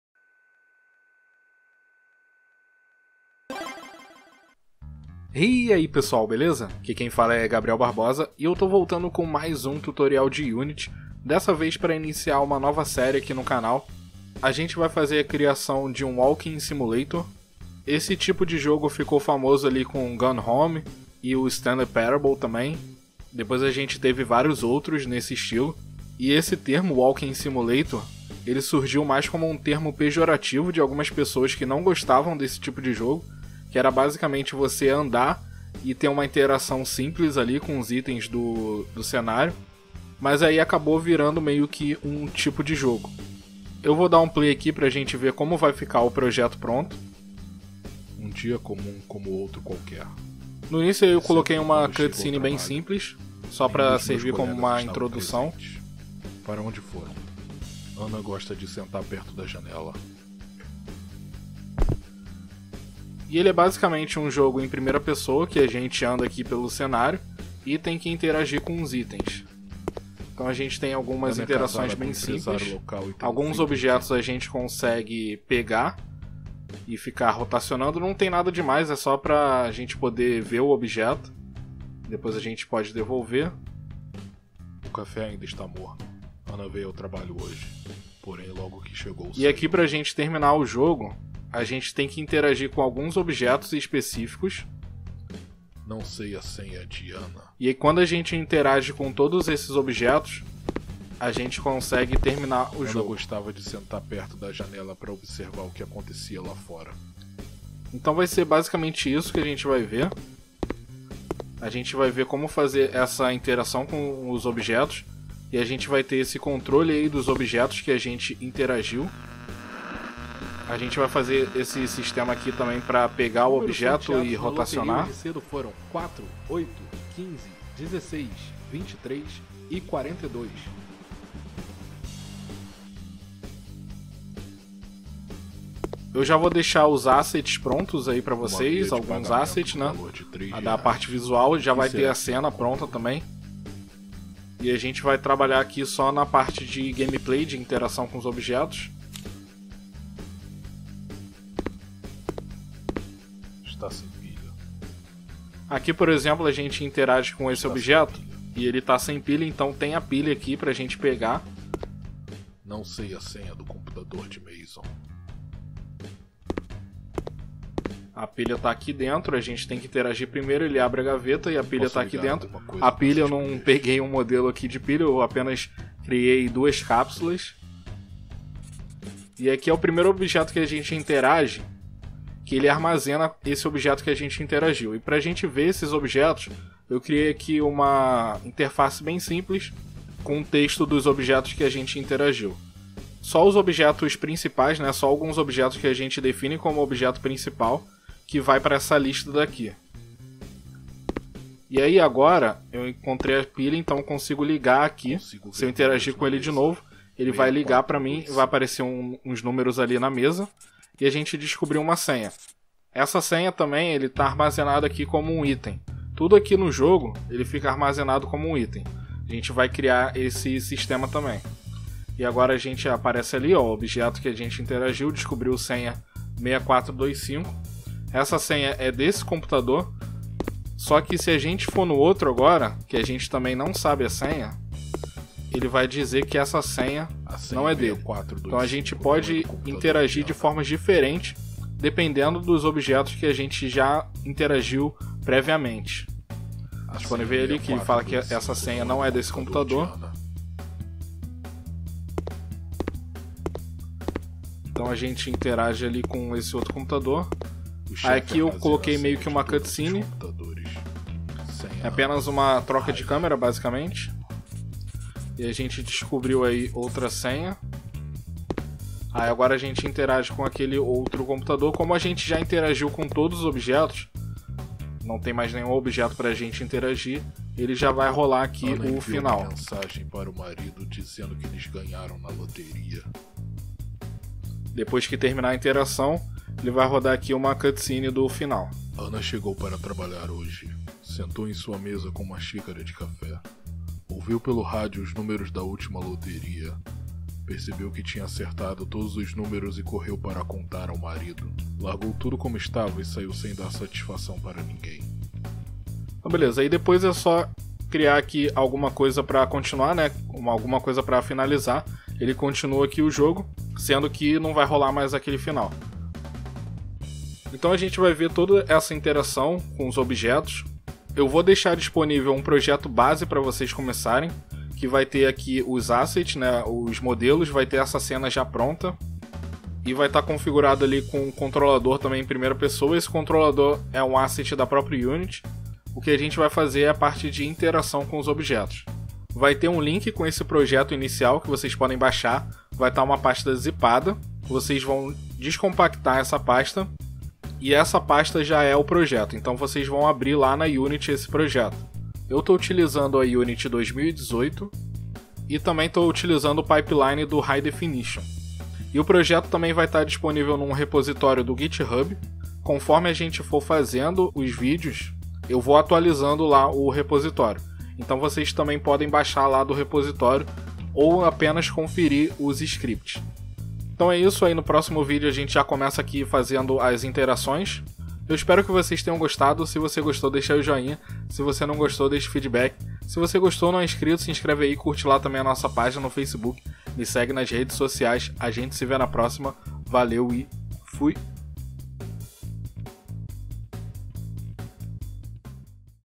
Walking Simulator na Unity | Introdução (E01)
Nessa série de tutorial vamos fazer a criação de um Walking Simulator. Nesse vídeo, uma introdução de como ficará o projeto finalizado.